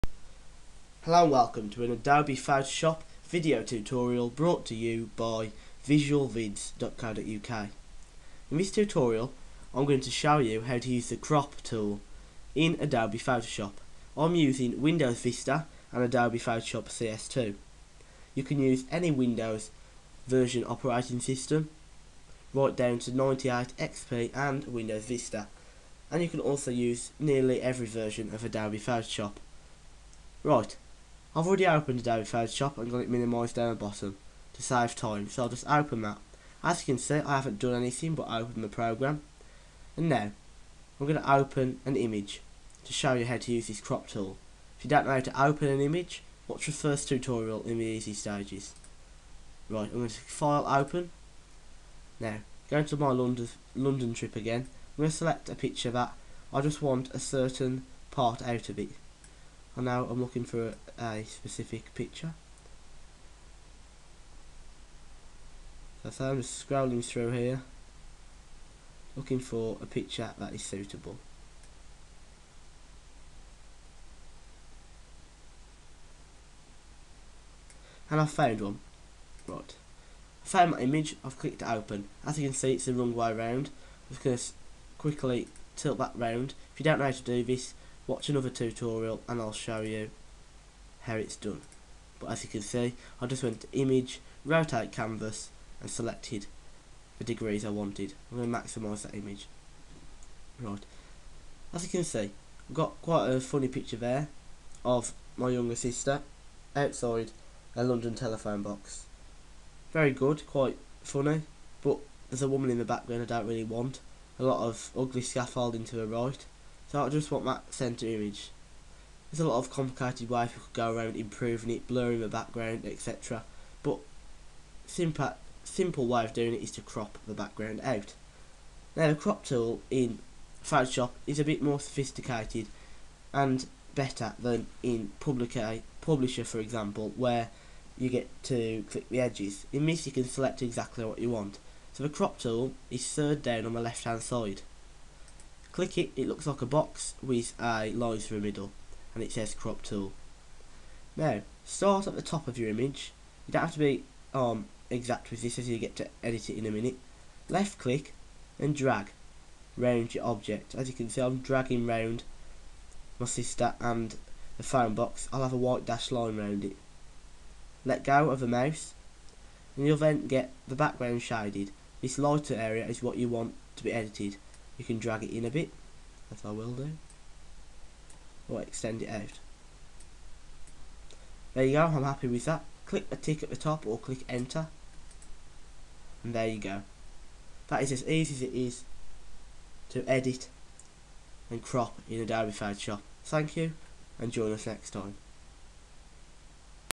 How to use the crop tool
learn how to use the crop tool in adobe photoshop
a free tutorial from visualvids.co.uk